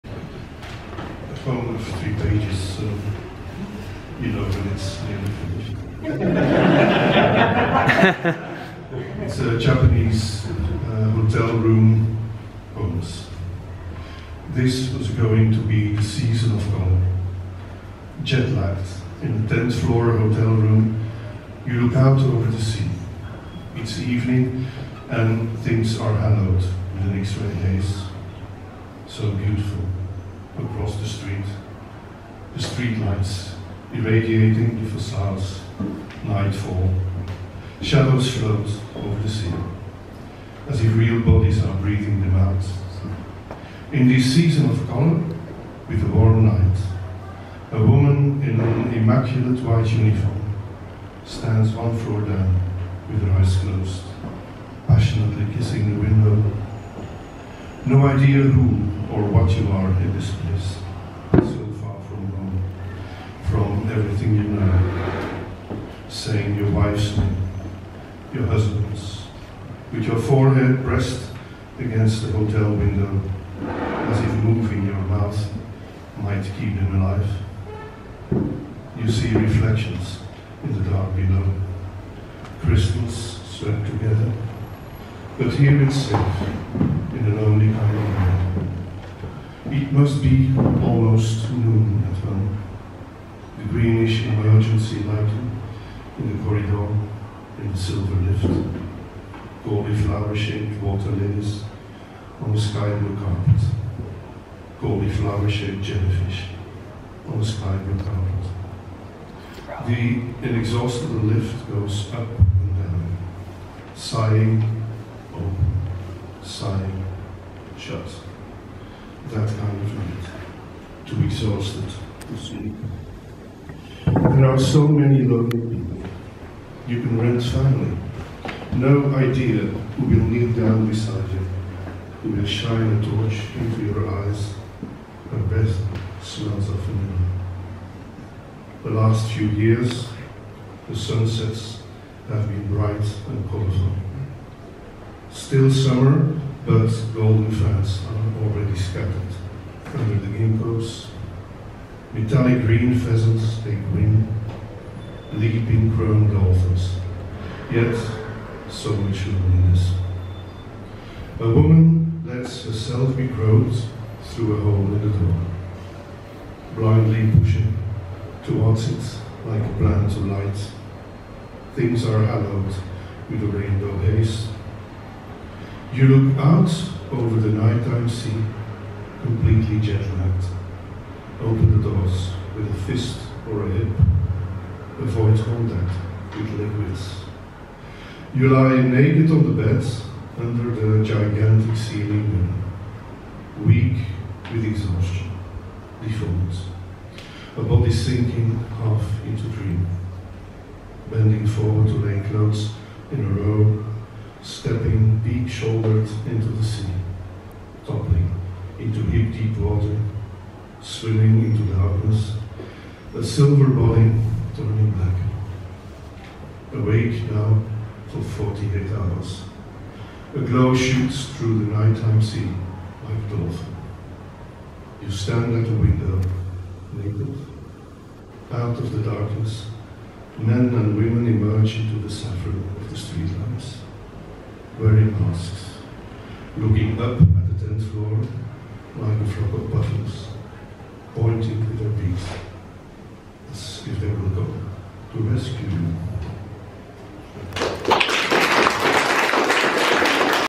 Peter Verhelst : Flanders - European Poetry Festival 2024
Flanders Poetry Celebration, June 20th 2024 at Flanders House

Three brilliant Flemish poets visited London to present brand new collaborations made with British counterparts just for this night. Celebrating the remarkable contemporary Flemish literary scene and exploring both literary and experimental poetry, this event was held in the beautiful environs of Flanders House, in the heart of London.

This event was generously supported by Flanders Literature and The Delegation of Flanders (Embassy of Belgium) to the UK.